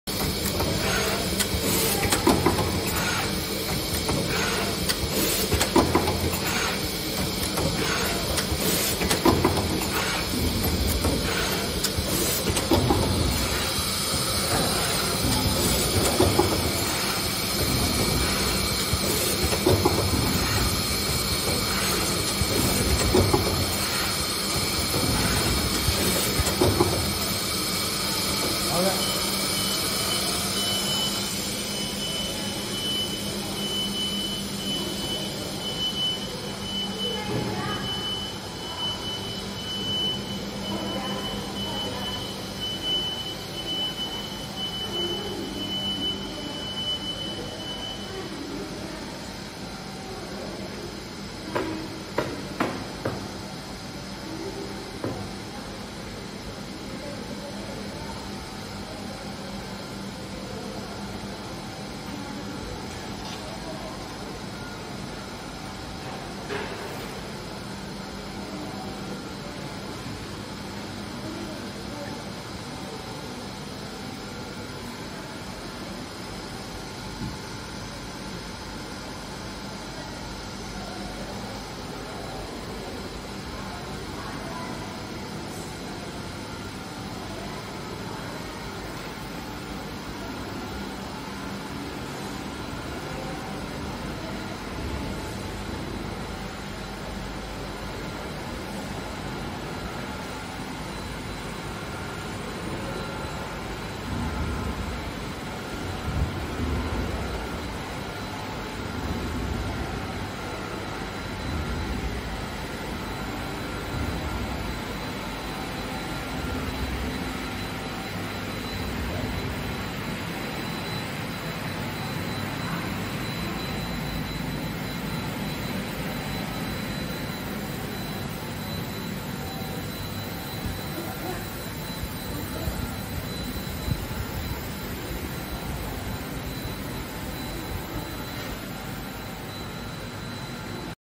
50MW solar panel manufacturing line
This is a 50MW solar panel manufacturing line for solar panel/ solar module.